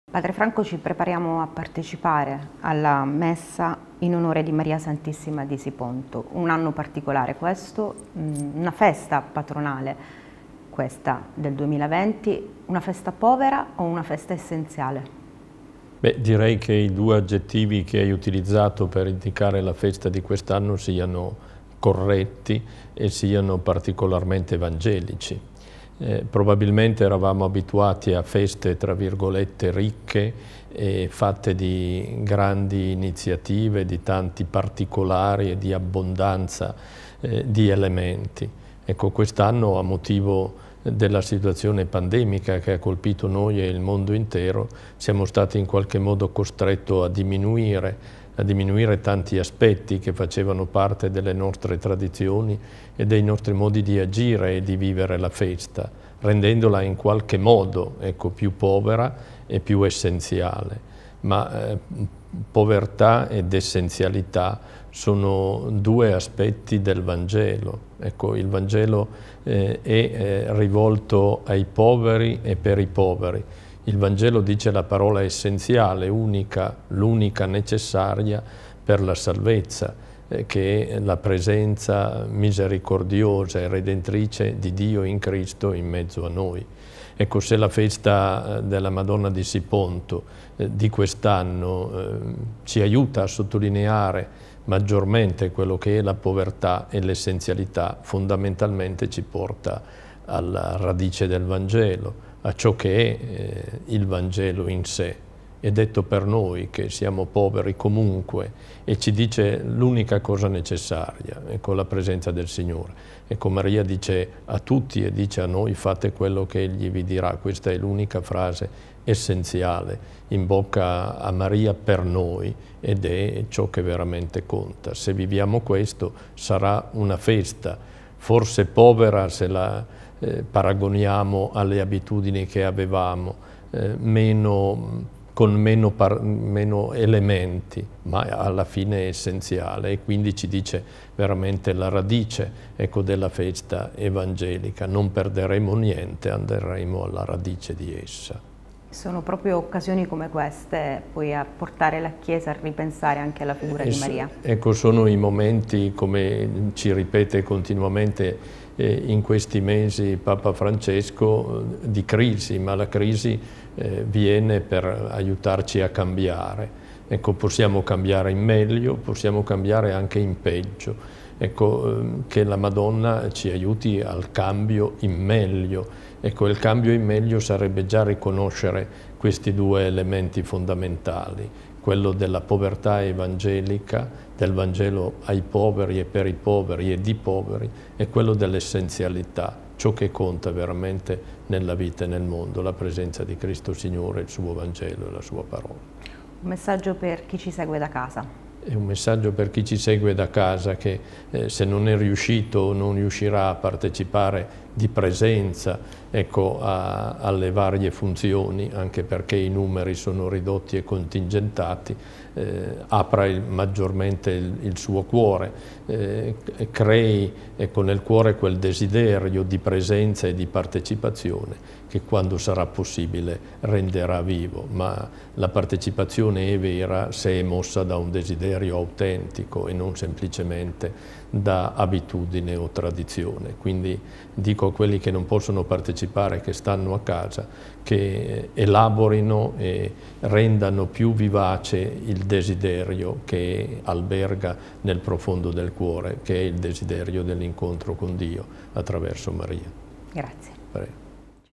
Intervista Padre Franco Moscone - Festa Patronale Manfredonia (30.08.2020)
Intervista a Padre Franco  Moscone per la  Festa Patronale della Madonna di Siponto del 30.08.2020.